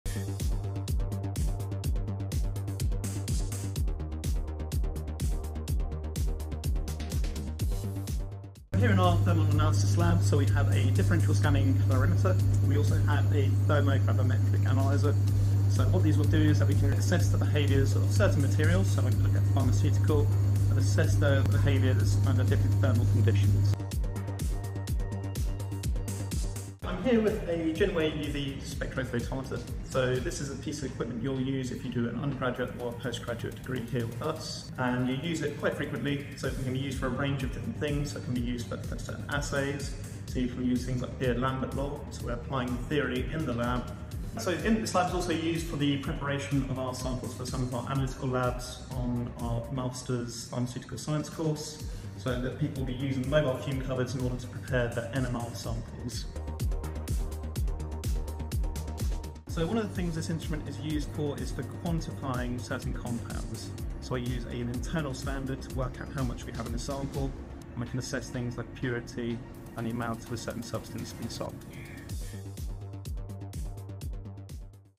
🔬 Medway Lab Tour – Apply Now!
Take a sneak peek inside our cutting-edge labs at the University of Greenwich Medway Campus! Want hands-on experience in real-world science? Applications are open now – don’t miss your chance to join us! 💡